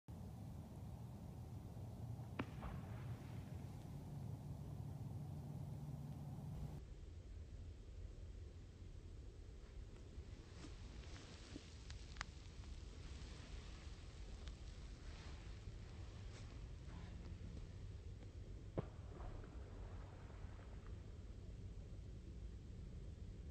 Beaver slap AUDIO. :03 & :19
(Not a gator) I was night fishing when an beaver started slapping not to far from me. They can be heard at :03 and :19. He probably did it thirty times or more total.